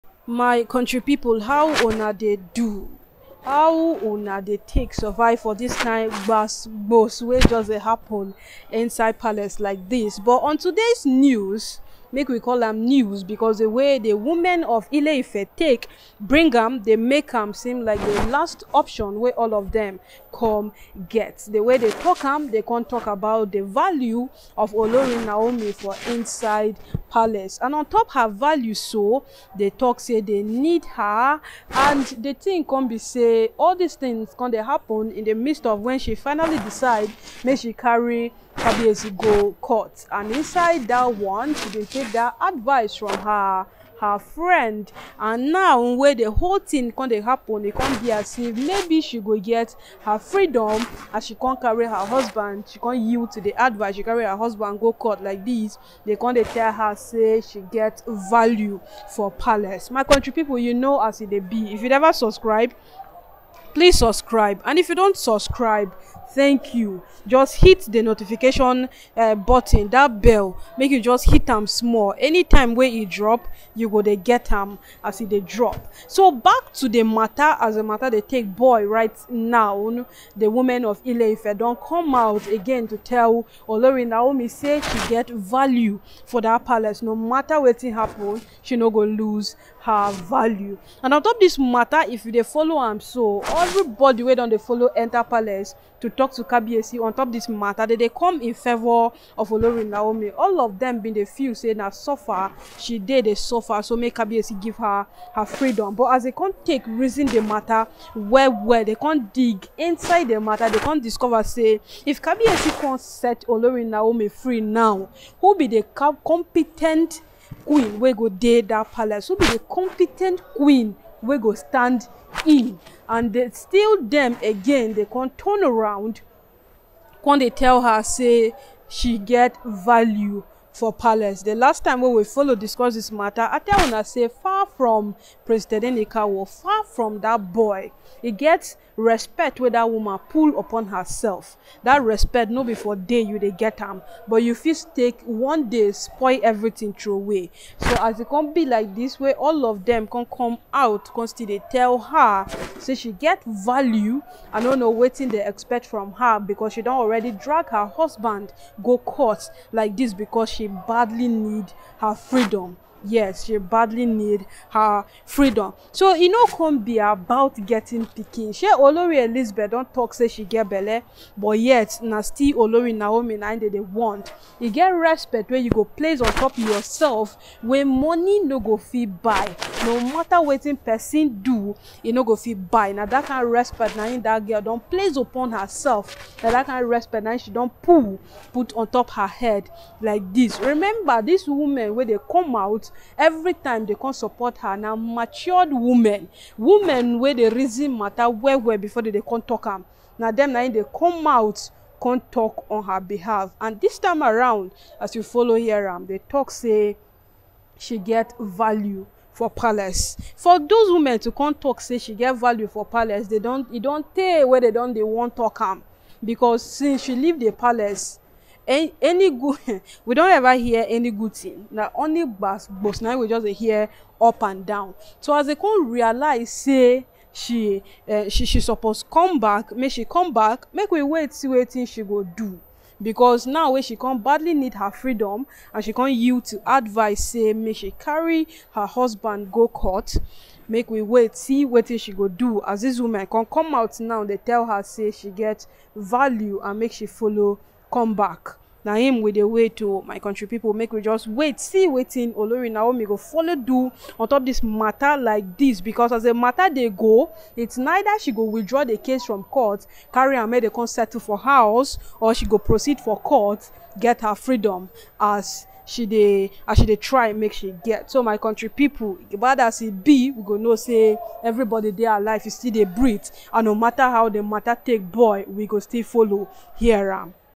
QUEEN NAOMI OGUNWUSI AT LOGGER HEADS WITH THE IFE WOMEN BECAUSE OF OONI BEHAVIOUR
Ashley tells Dr lizzy to focus on making babies

Timestamps
0:00 Intro Ooni of ife speak to wives
0:10 ooni of ife break silence on queen naomi
1:15 ooni of ife and olori naomi break silence
1:16 ooni of ife queen naomi speak ooni
3:15 olori mariam
3:20 ooni of ife queen naomi engaged
3:25 olori tobi phillips
3:30 olori Dr Elizabeth
4:00 olori Temitope adesegun
4:02 olori Ashley adegoke
4:03 olori Ronke

yeye kudi.

it's over oon of ife hang bag oonipalace ooni abikejagaban mariam anako finally shock

Ashley Ex Husband kn palace to collect his child.

Queen naomi annual jubilation praise oloriashley oonibrother yeyekudi